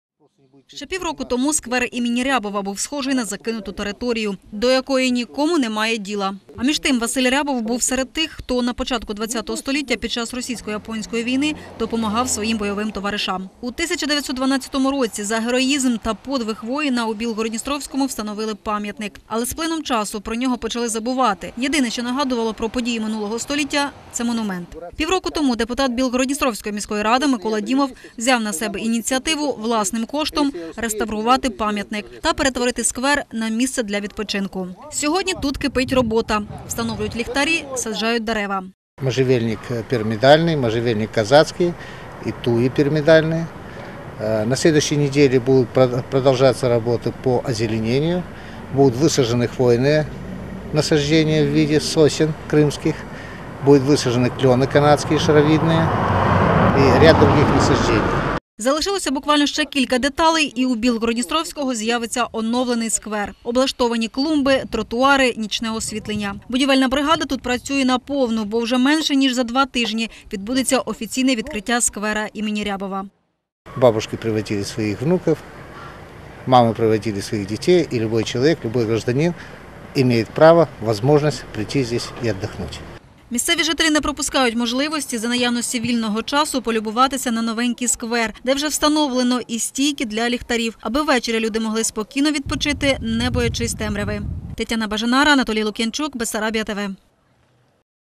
Сквер ім. Рябова майже готовий
У Білгород-Дністровському більшає зон для відпочинку. Вже за два тижні аккеманці зможуть прогулятися новим сквером. Відреставровані тротуарні доріжки, новенькі лавки для сидіння та клумби європейського зразку -  все це вже майже встановлено у сквері ім. Рябова. Подробиці  -  у сюжеті